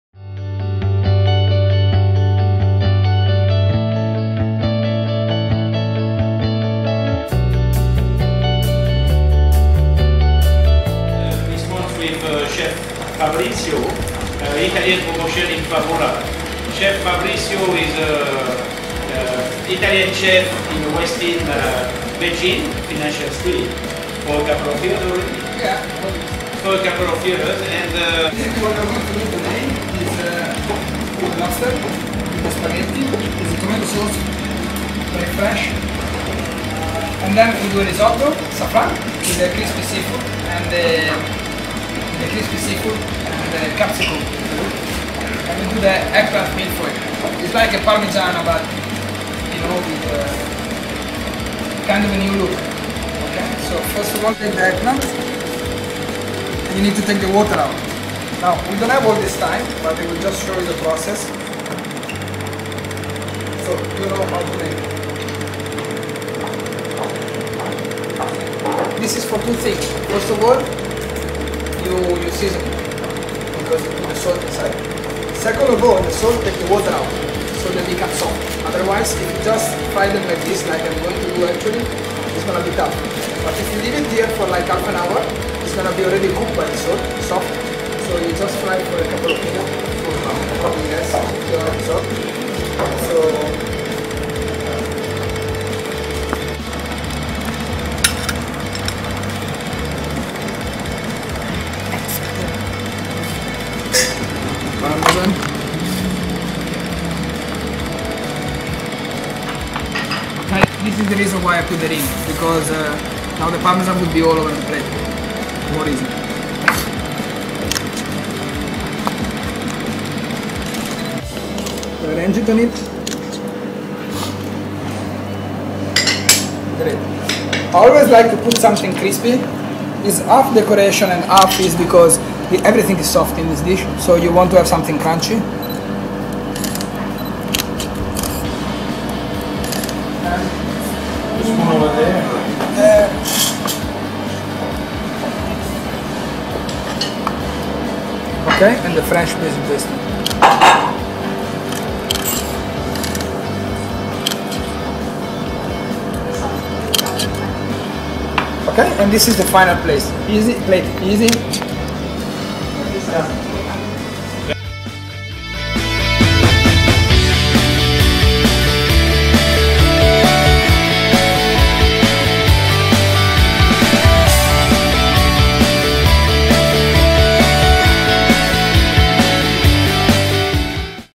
Inspiring Italy: Eggplant Millefeuille by Chef Fabrizio
A cooking demo of how to make a
Modern interpretation of the classic Eggplant Parmagiana in conjunction with Le Meridien Kuala Lumpur's Italian Promotion at Favola